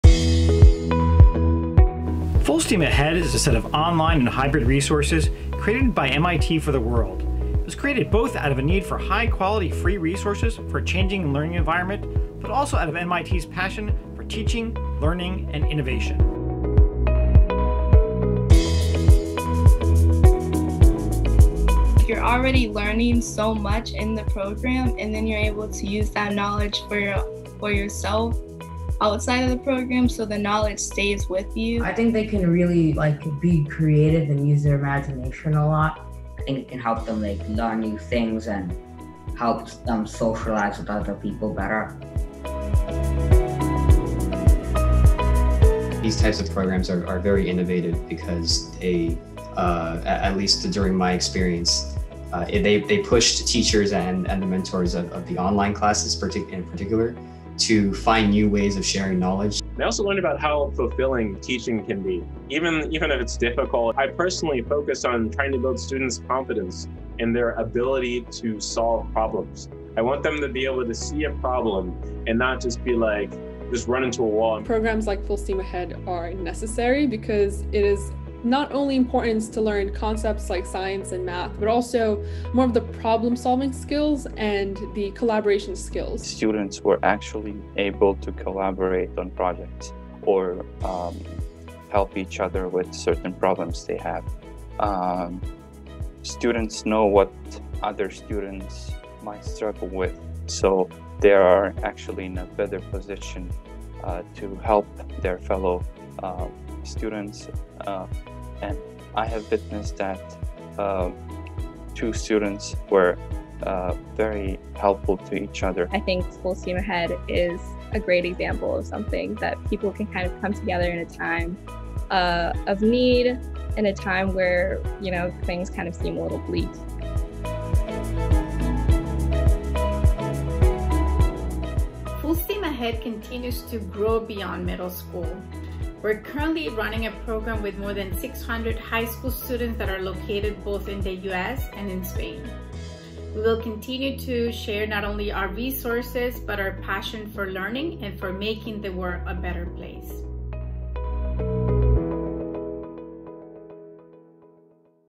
MIT Full STEAM Ahead
MIT Full STEAM Ahead is a collection of online teaching and learning activities, tools, and tutorials, gathered as a rapid response to the need for online resources during the drastic disruption to education caused by the COVID-19 pandemic. A second phase of the project, Full STEAM Ahead into Summer, began at the close of the past academic year, consisting of a virtual summer program and academic enrichment opportunity for middle school students that combines hands-on exploration, project design, and skill building in STEM subjects.